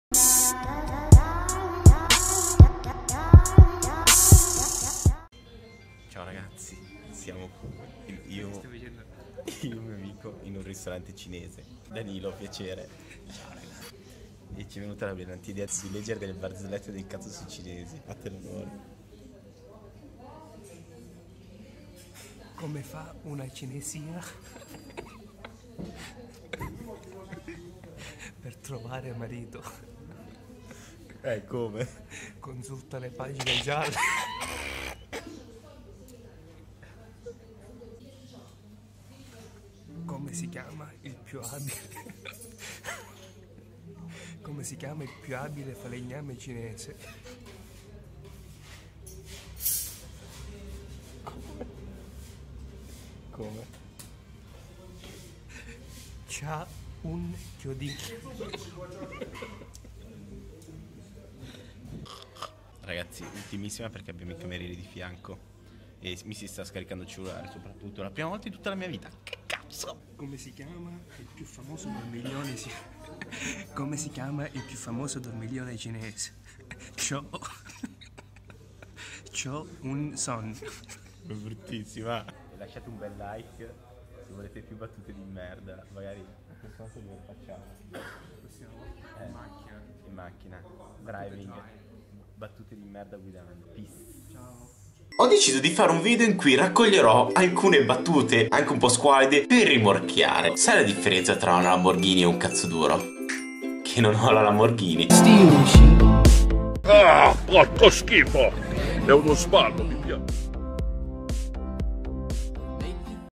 Barzellette SQUALLIDE sui cinesi in un ristorante cinese|THE PETRI
Ragazzi premetto che il video è stato completamente improvvisato durante una cena in un ristorante cinese, ma spero di riuscirvi a strappare comunque un sorriso,se vi è piaciuto lasciate un like ed iscrivetevi!